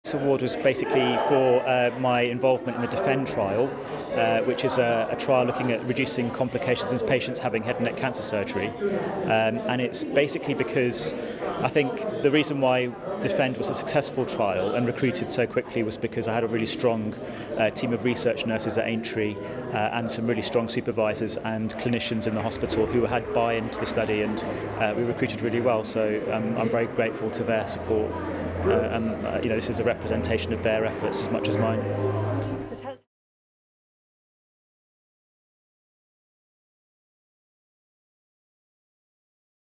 Research Student of the Year
Mandeep S. Bajwa from the University of Liverpool and Liverpool Head and Neck Centre reacts to being named Research Student of the Year at the 2020 North West Coast Research and Innovation Awards.